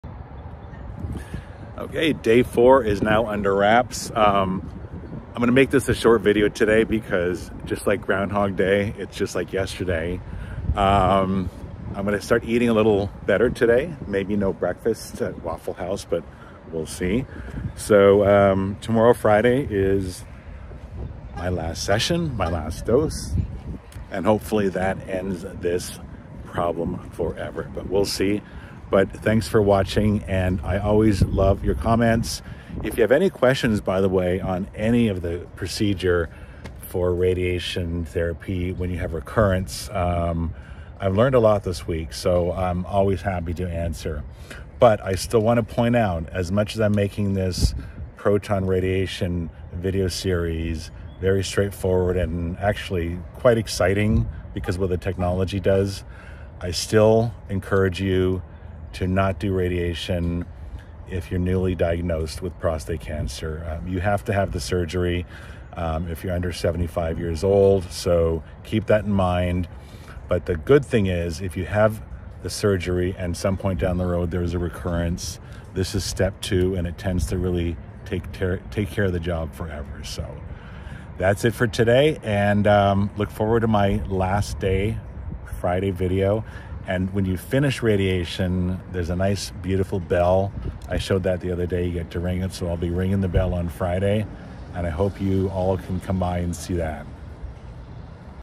Prostate Cancer Recurrence - Day 4 of My Proton Radiation Treatment
It's Day 4 of 5...and not much to report other than things are going well.  I'm almost there, folks.  Watch my full series to understand my journey with prostate cancer that started in 2015, and recurred in 2023.